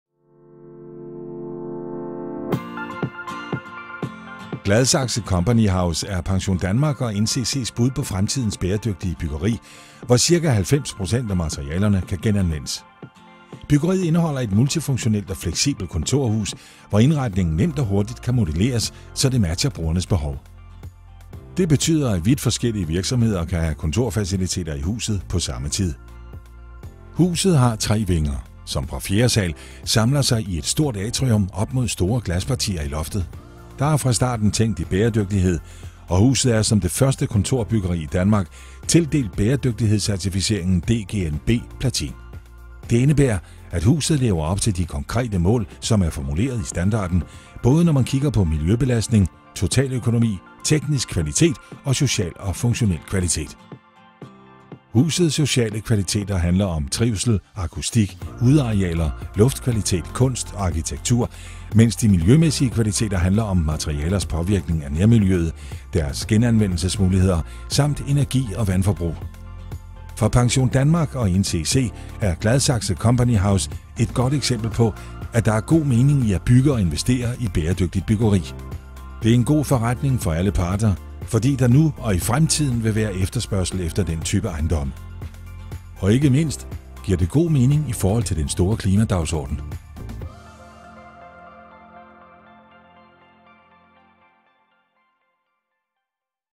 Gladsaxe Company House - bæredygtigt kontorhus i særklasse
Gladsaxe Company House, der har opnået topkarakteren Platin inden for bæredygtighedsnormen DGNB, er et eksempel på NCC’s ambitioner inden for bæredygtighed.

PensionDanmark ejer Gladsaxe Company House, men huset er udviklet og bygget af NCC, og rummer blandt andet NCC´s danske hovedkontor.